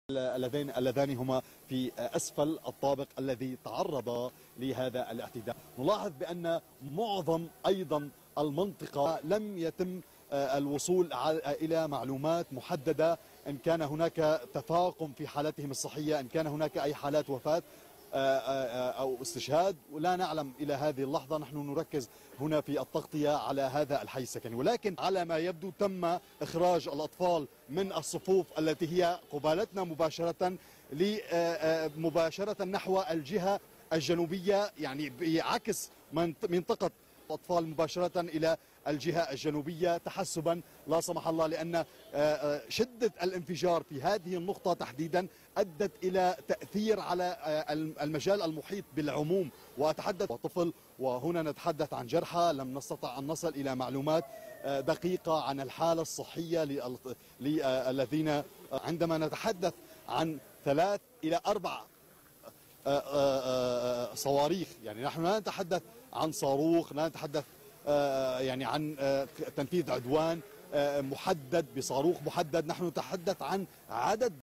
Siria, raid israeliani su un quartiere residenziale di Damasco: almeno tre morti
Media, ucciso un passante oltre a due persone non siriane